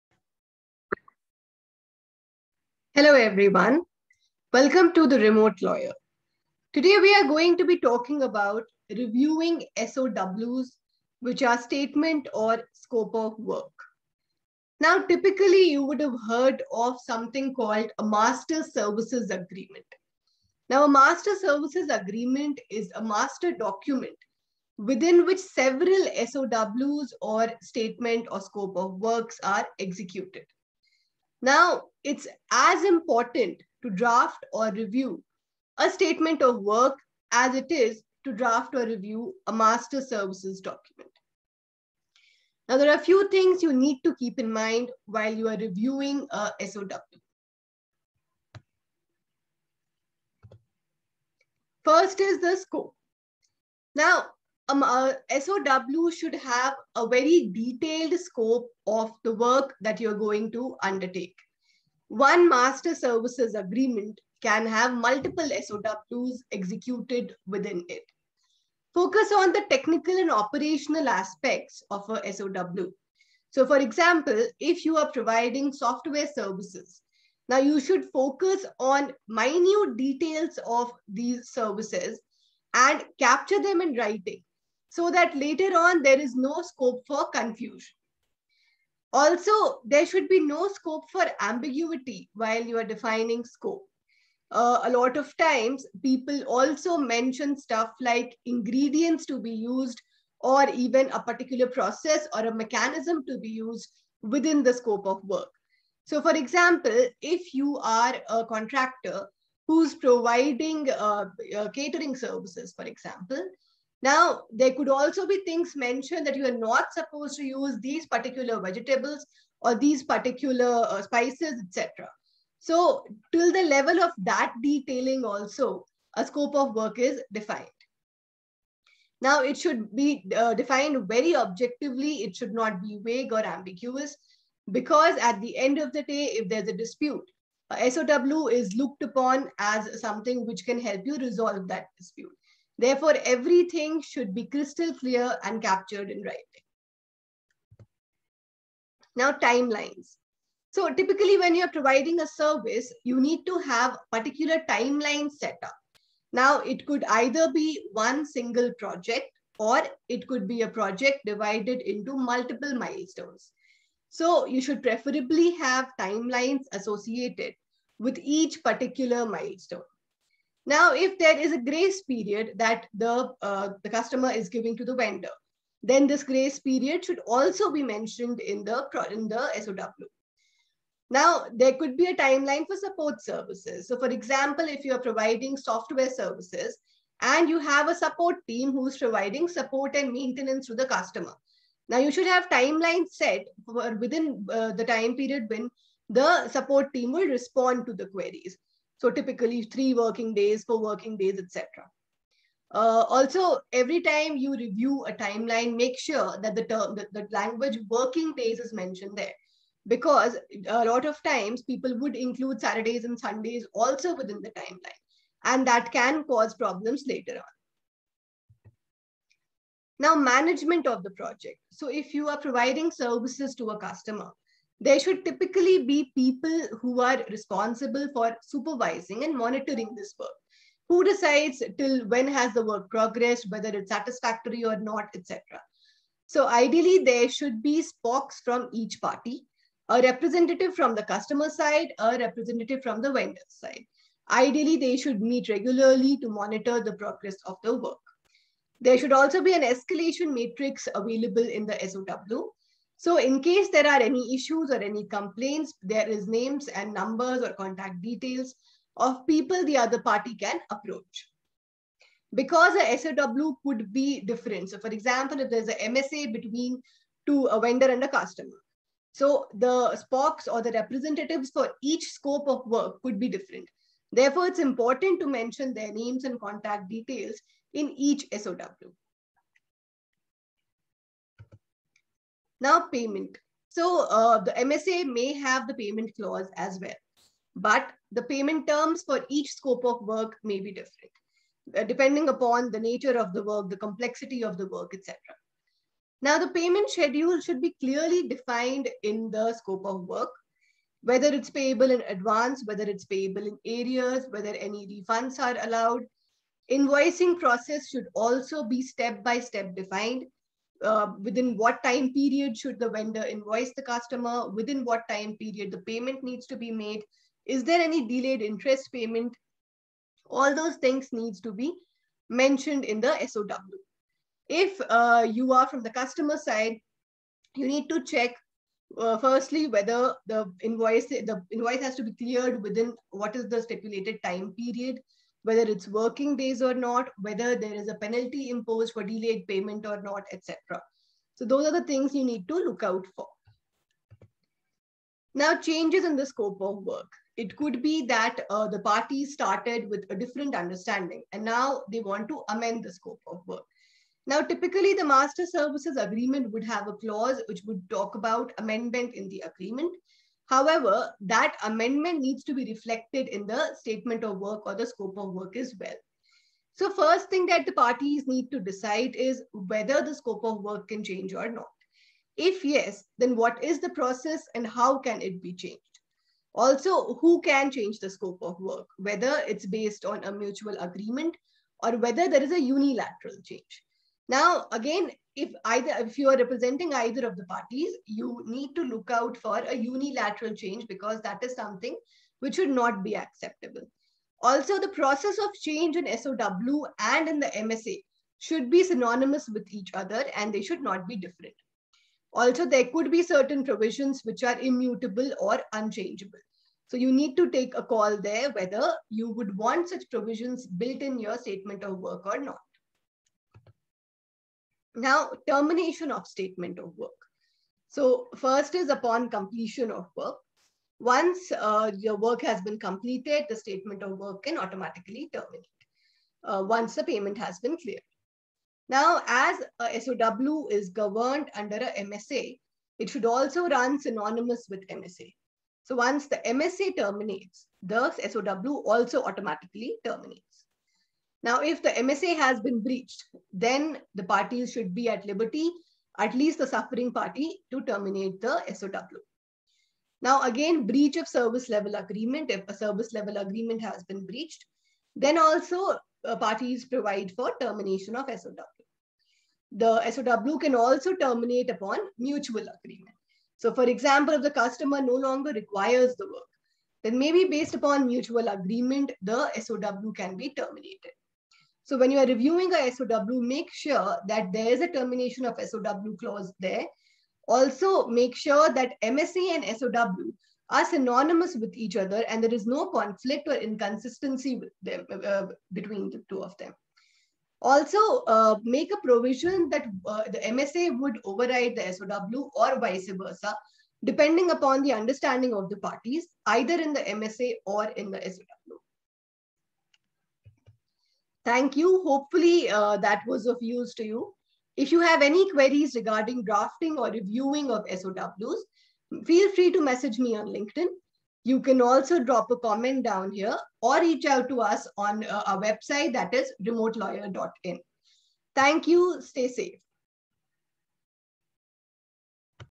Reviewing SOW: Things to Look Out For!
Every Master Services Agreement has a SOW. Let us find out what all needs to be included in a SOW and how to review the same.